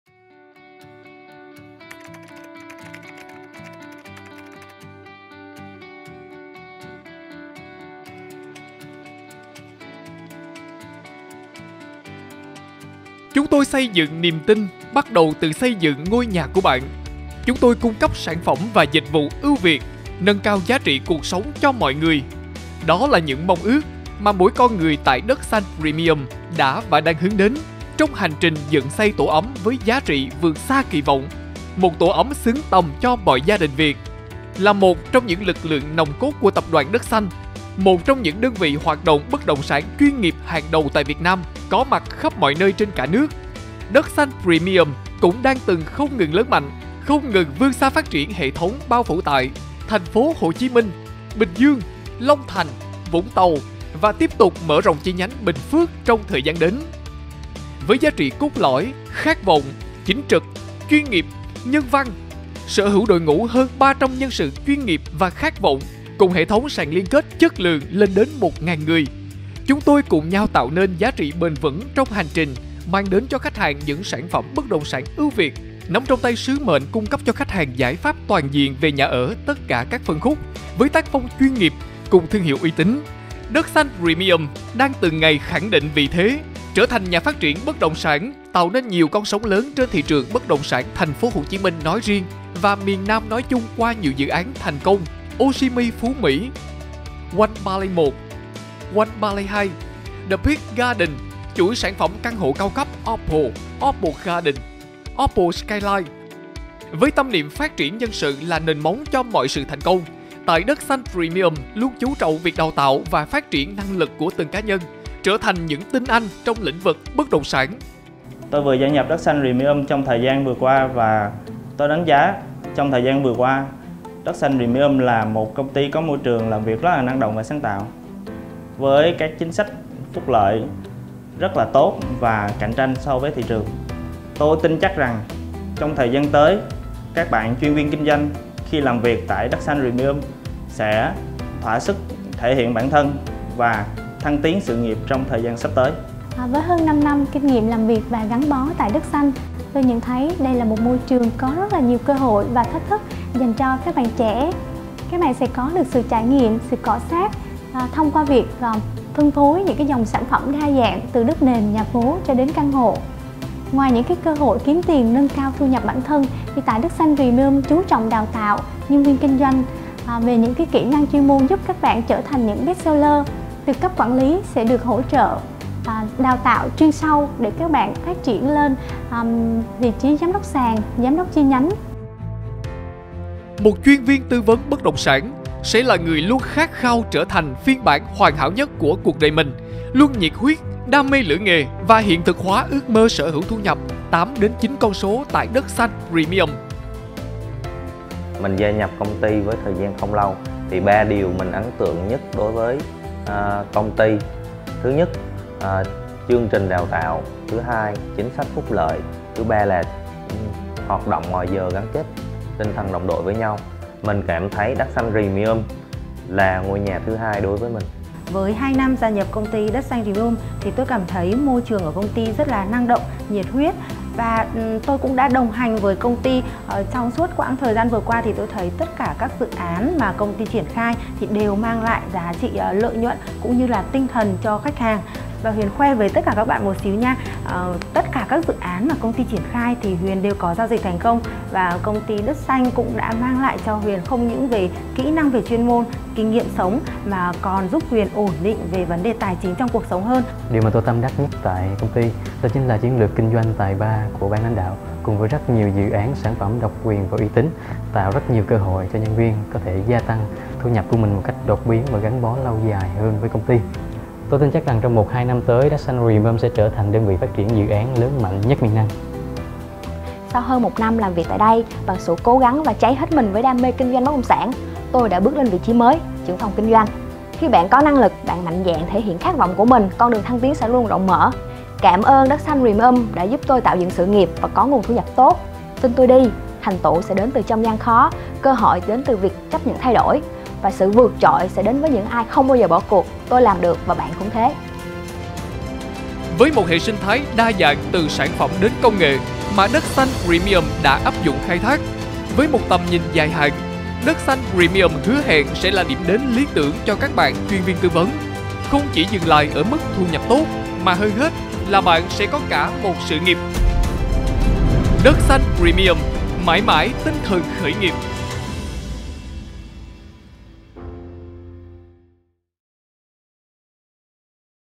Phát Triển Sự Nghiệp tại Dat Xanh Premium
Để đáp ứng cho sự phát triển lớn mạnh của hệ thống kinh doanh Hội sở Dat Xanh Premium và các công ty thành viên trong năm 2022, Dat Xanh Premium chào đón những nhân sự đầy nhiệt huyết, sẵn sàng dấn thân và luôn gắn suy nghĩ vào trong từng mục tiêu gia nhập hệ thống kinh doanh chất lượng cao tại Dat Xanh Premium.
Đến với Dat Xanh Premium, không chỉ dừng lại ở mức thu nhập tốt mà hơn hết bạn sẽ có cả một sự nghiệp.